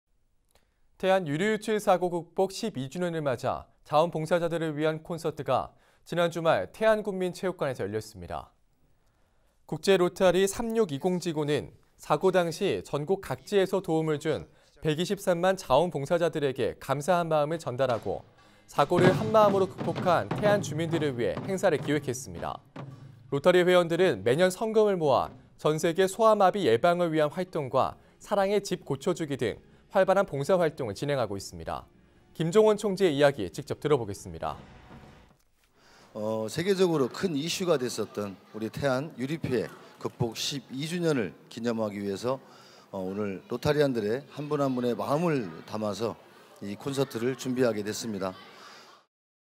태안 유류피해극복 12주년 콘서트 열려
태안 유류유출사고 극복 12주년을 맞아 자원봉사자들을 위한 콘서트가 지난 주말 태안군민체육관에서 열렸습니다.